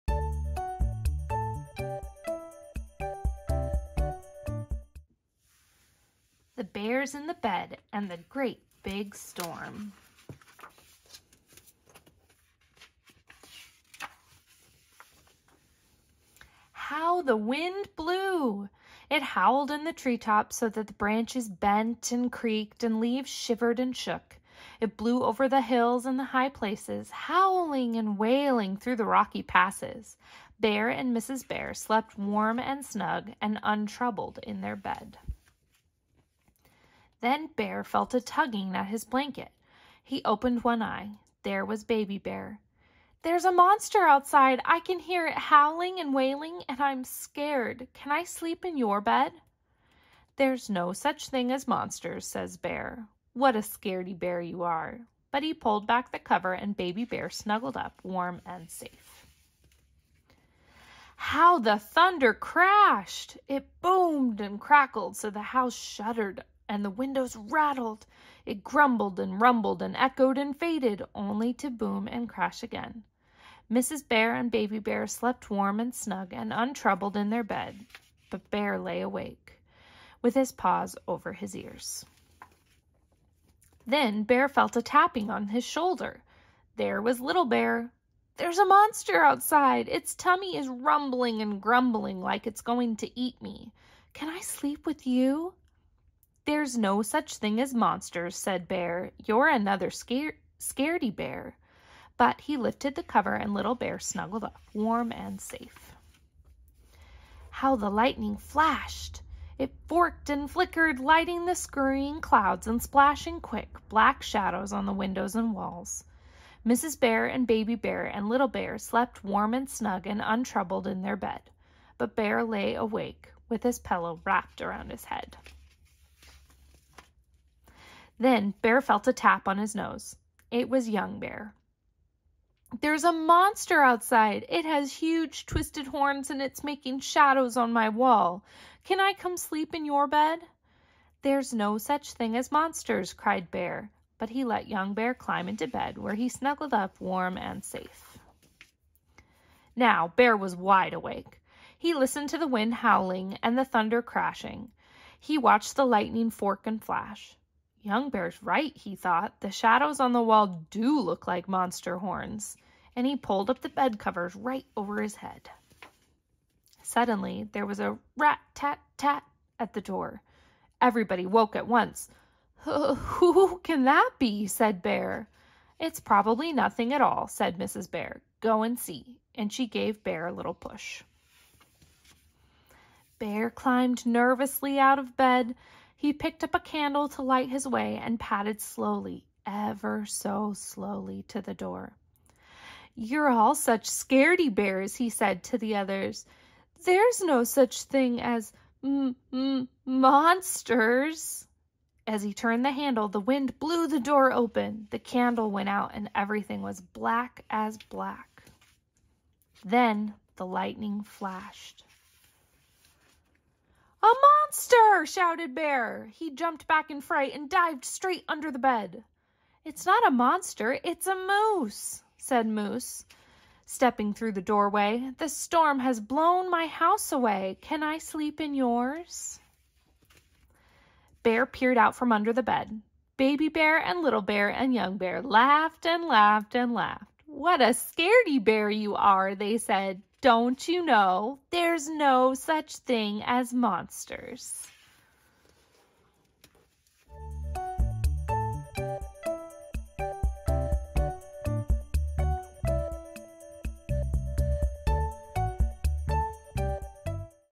Let's Read a Book Aloud! The Bears in the Bed and the Great Big Storm
The Bears in the Bed and the Great Big Storm by Paul Bright

A family of bears is trying to sleep, but a storm is keeping them up! The young scaredy bears climb in bed with mom and dad, but then dad gets a case of the scaries as well! Turns out, thunder is nothing to be scared of, but it is awfully loud!

Have a favorite book you haven't seen us read yet? Let us know!